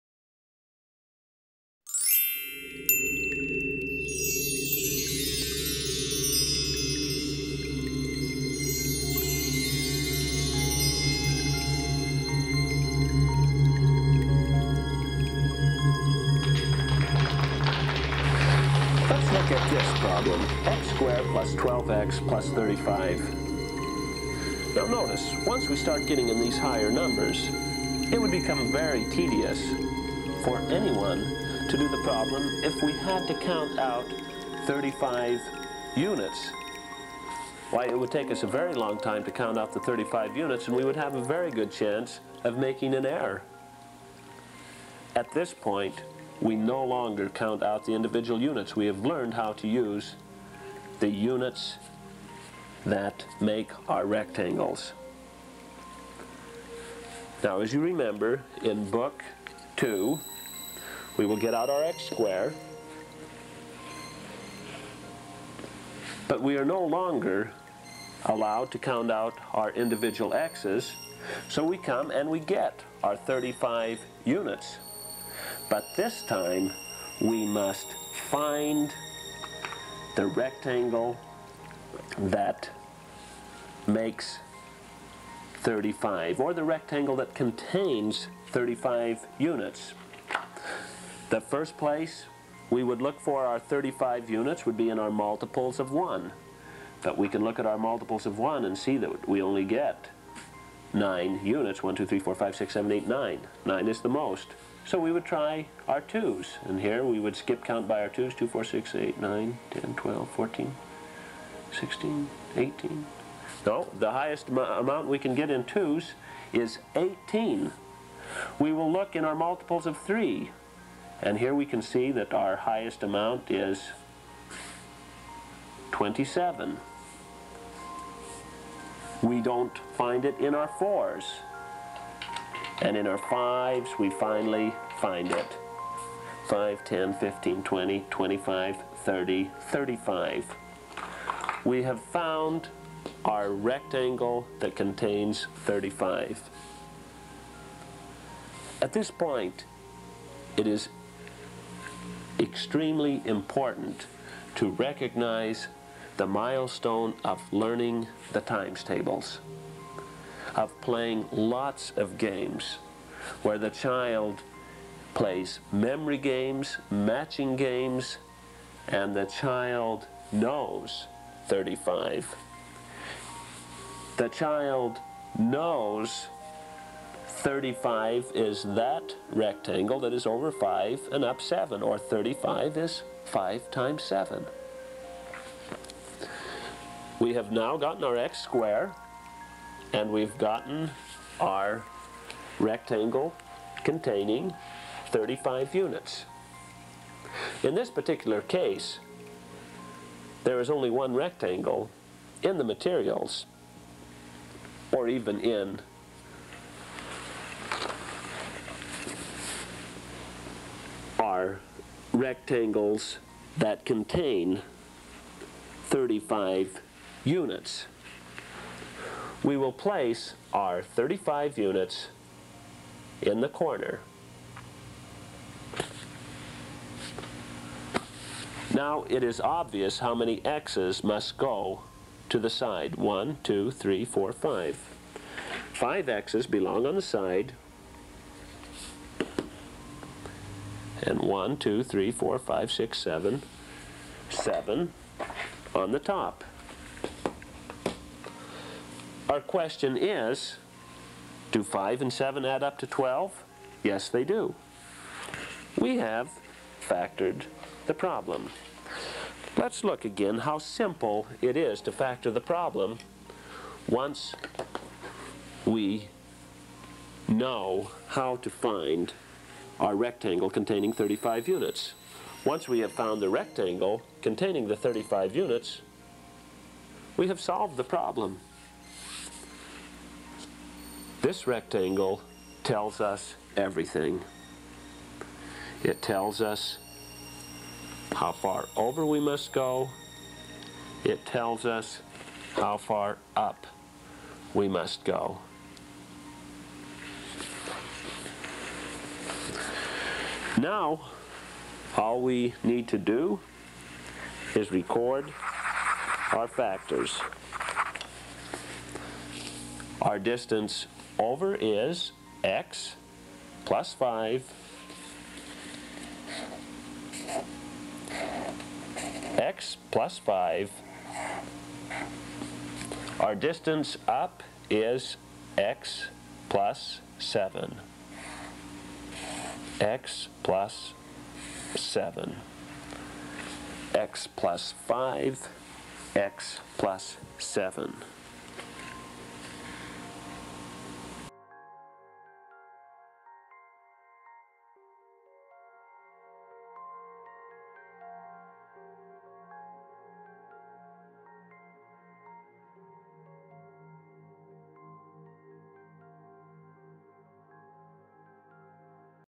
Factoring Expressions, Mortensen Math.com, Kids Montessori K-12 Homeschooling tutorial video
Factoring Expressions, with Jerry Mortensen, Mortensen Math manipulative blocks, Kids Montessori K-12 Homeschooling Pre-school Kindergarten mathematics tutorial video.\